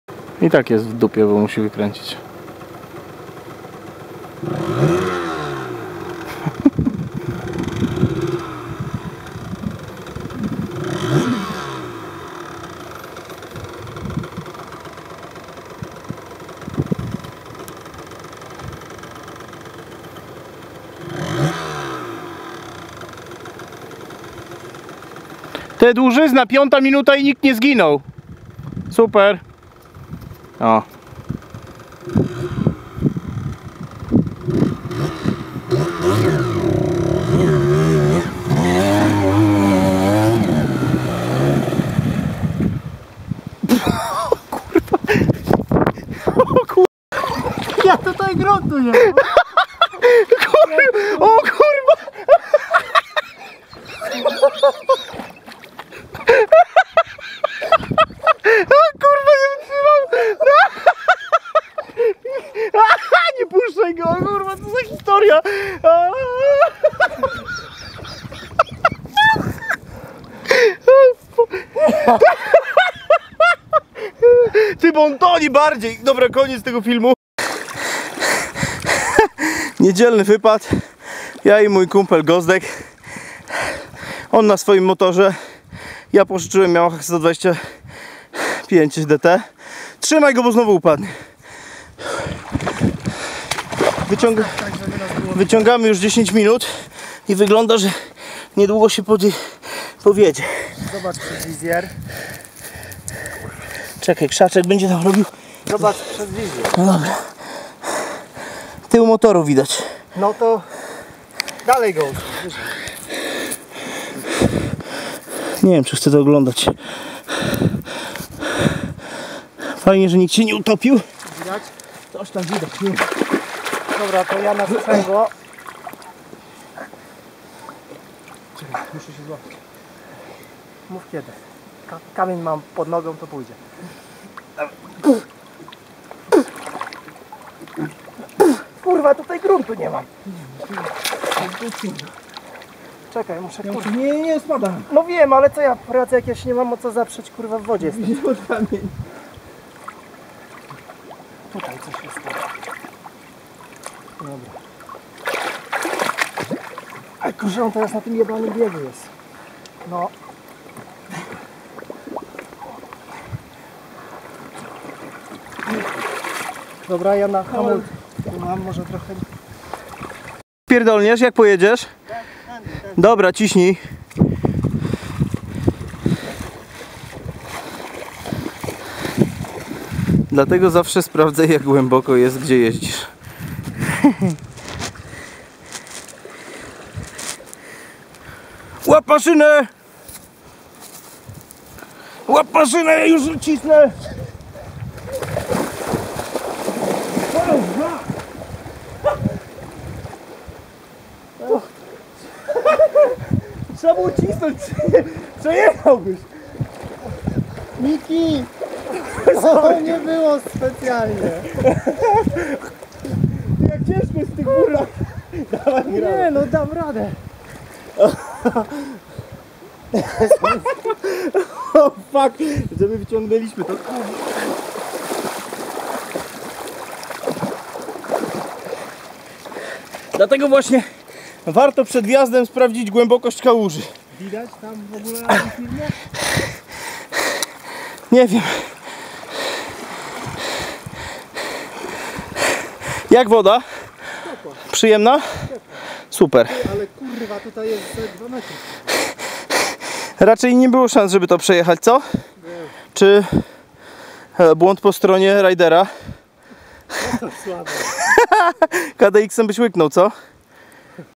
Yamaha DT125 - to byla gleboka kaluza
Miki... chyba zepsulem Ci motor...

Historia byla taka, ze z Szopa (ten co krecil film, pozyczyl od Mikiego DT ktora utopilem) pojechalismy troche poszalec. Niestety po kilku kilometrach w moim Kawasaki KDX pekla detka wiec postanowilismy uratowac weekend i pojezdzic na DT po  wodzie w znajomej minikopalni.

Skonczylo sie jak widac :) Wlasciciel wykopal tam dol o wymiarach ok 3mx5m i glebokosci ~2m :) Nie bylo go widac przez wode. Kilka minut wczesniej z 10 razy przejezdzalismy tamtedy (jak sie potem okazalo doslownie kilkanascie cm od skarpy) ale akurat ja mialem zaszczyt utopic maszyne :)

Bylo dosc gleboko i chwilami nie mialem gruntu... trzymalem DT za kierownice zeby nie polozyla sie na dnie bo by byla kaplica calkowita juz i raczej bysmy jej nie wyciagneli :)

No coz, zdarza sie :) Kupa smiechu i pare stow do tylu :)